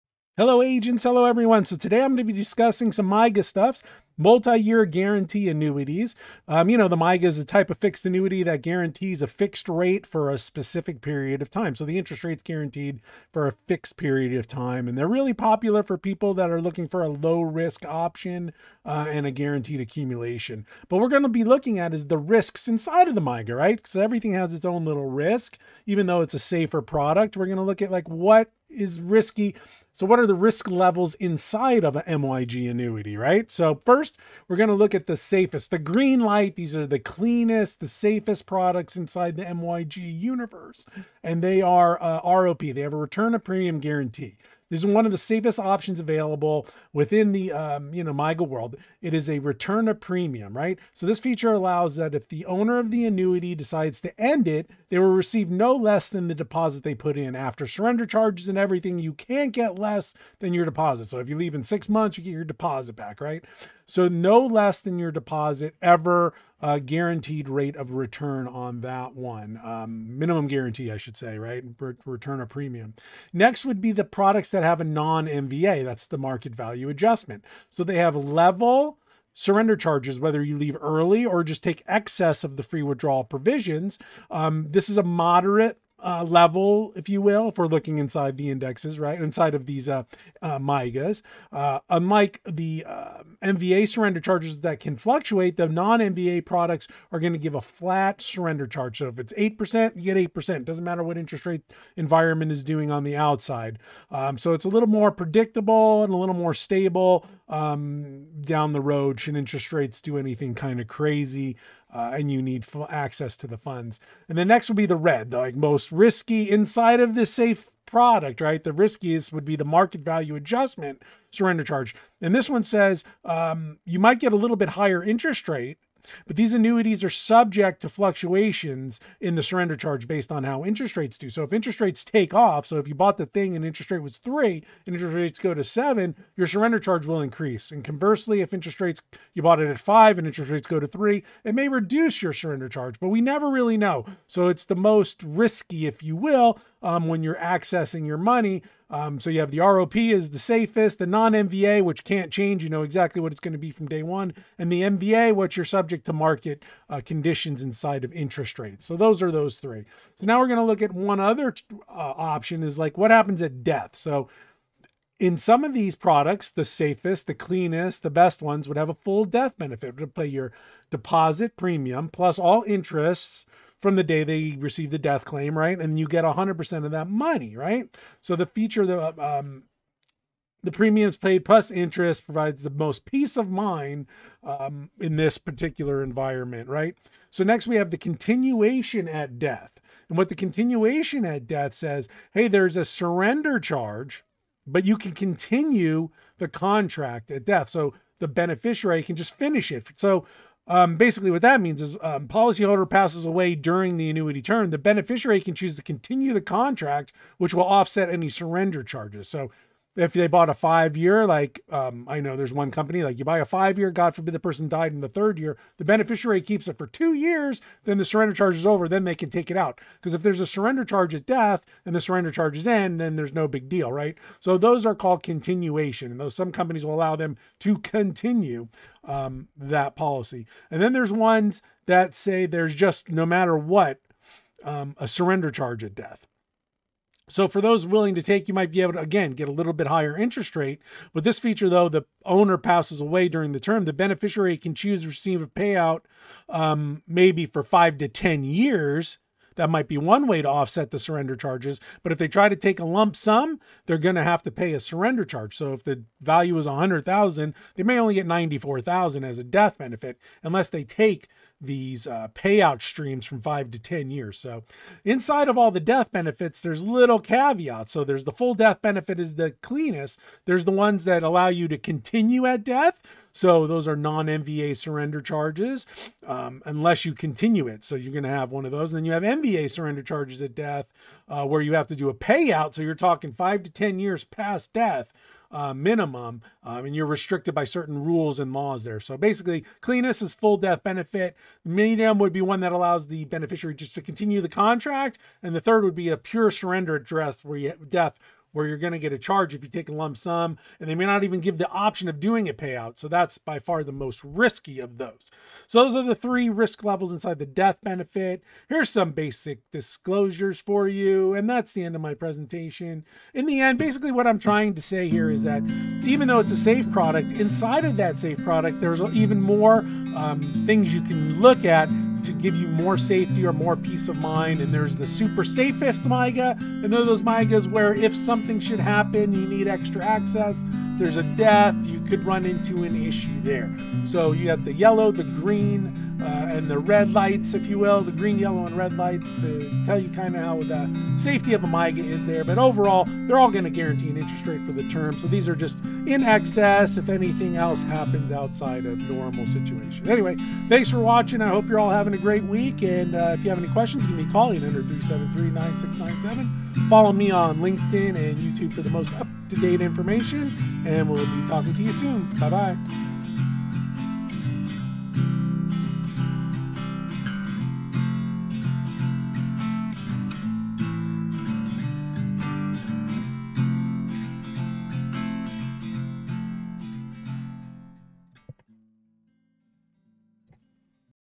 An Overview of Risk Levels in MYG Annuities. ROP, surrender charge, death benefit & Market Value Adj
An Overview of Risk Levels in MYG Annuities

Early or Excess Withdrawal Risk:
Green Level: Offers Return of Premium (ROP) with no surrender charges.

Yellow Level: Offers a non-Market Value Adjustment (non-MVA) surrender charge.

Red Level: Offers Market Value Adjustment (MVA) surrender charges.

Death Benefit Risk:
Green Level: Provides full death benefit.

Yellow Level: Provides continuation or non-MVA surrender on lump sum death claim.

Red Level: Offers 5-10 year annuitization or surrender charge +/- MVA on death claims